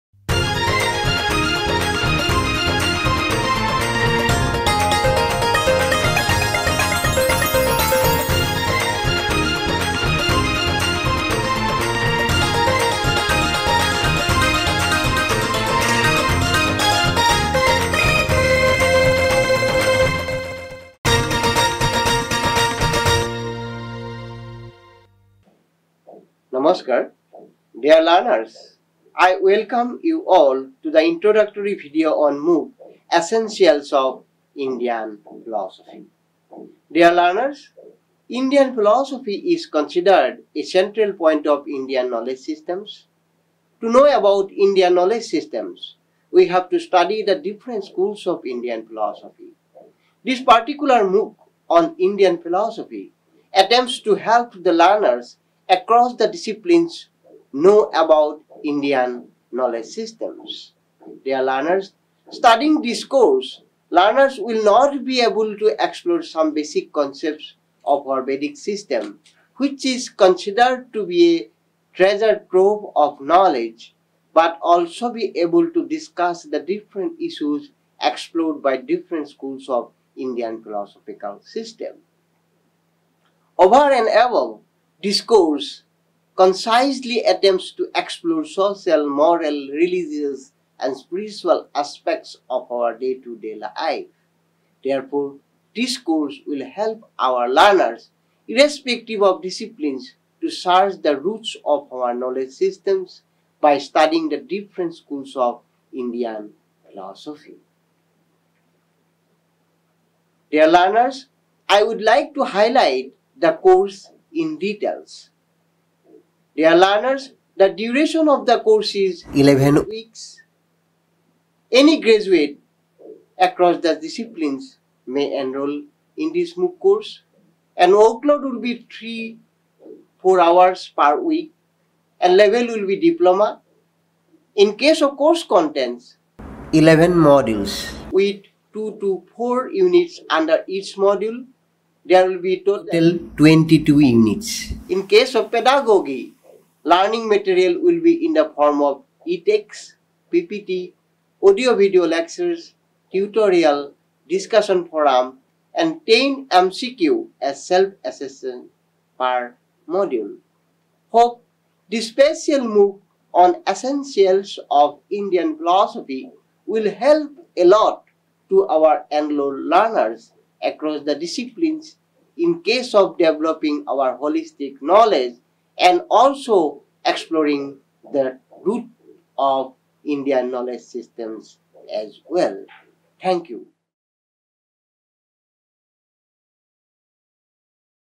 MOOC ON ESSENTIALS OF INDIAN PHILOSOPHY
Dr. Bhaskar Bhattacharyya
Assistant Professor, Dept.of Philosophy,
Surya Kumar Bhuyan School of Social Sciences
KKHSOU